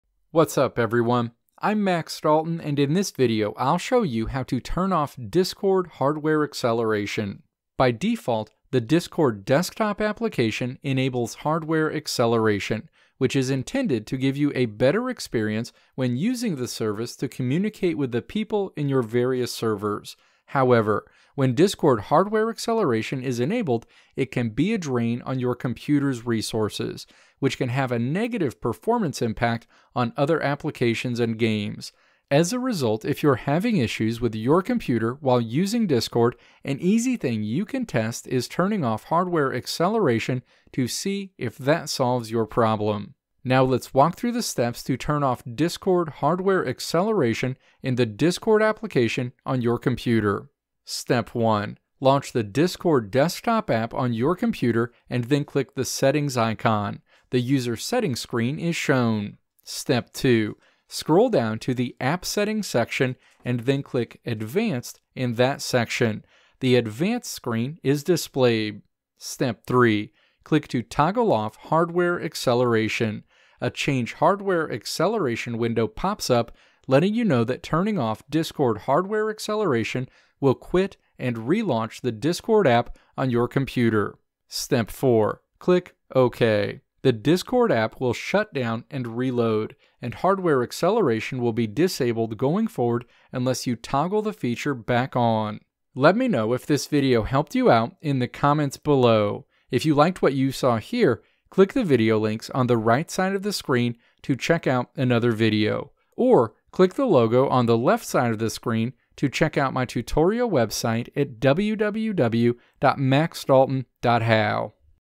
How to Turn Off Discord Hardware Acceleration
In this video I'll show you how to turn off Discord hardware acceleration.

Timestamps:
Introduction: 0:00
Steps to Turn Off Discord Hardware Acceleration: 0:43
Conclusion: 1:35

Video Transcript:
By default, the Discord desktop application enables hardware acceleration, which is intended to give you a better experience when using the service to communicate with the people in your various servers. However, when Discord hardware acceleration is enabled it can be a drain on your computer's resources, which can have a negative performance impact on other application's and games. As a result, if you're having issues with your computer while using Discord, an easy thing you can test is turning off hardware acceleration to see if that solves your problem.

Now let's walk through the steps to turn off Discord hardware acceleration in the Discord application on your computer.

Step 1. Launch the Discord desktop app on your computer and then click the "Settings" icon. The User Settings screen is shown.

Step 2. Scroll down to the "App Settings" section, and then click "Advanced" in that section. The Advanced screen is displayed.

Step 3. Click to toggle off "Hardware Acceleration." A Change Hardware Acceleration window pops up, letting you know that turning off Discord hardware acceleration will quit and re-launch the Discord app on your computer.

Step 4. Click "Okay." The Discord app will shut down and reload, and hardware acceleration will be disabled going forward unless you toggle the feature back on.